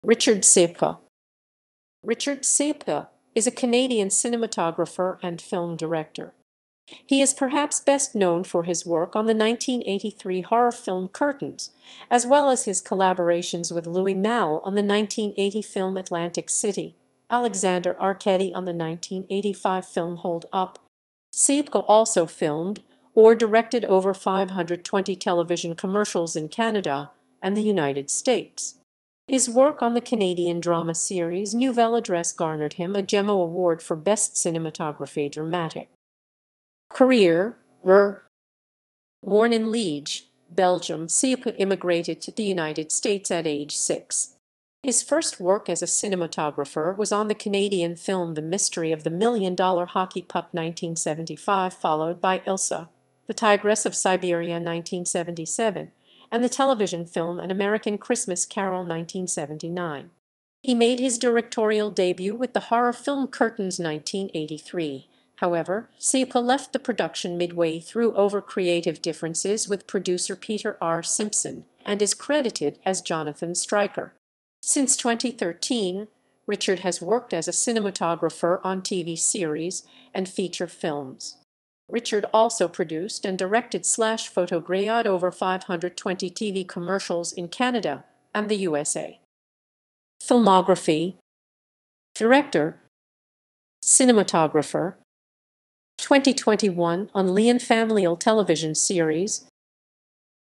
Richard Ciupka
Richard Ciupka is a Canadian cinematographer and film director. He is perhaps best known for his work on the 1983 horror film Curtains, as well as his collaborations with Louis Malle on the 1980 film Atlantic City, Alexandre Arcady on the 1985 film Hold-Up and Claude Chabrol on three feature films in France. Ciupka also filmed or directed over 520 television commercials in Canada and the United States. His work on the Canadian drama series Nouvelle adresse garnered him a Gémeaux award for "Best Cinematography (Dramatic)."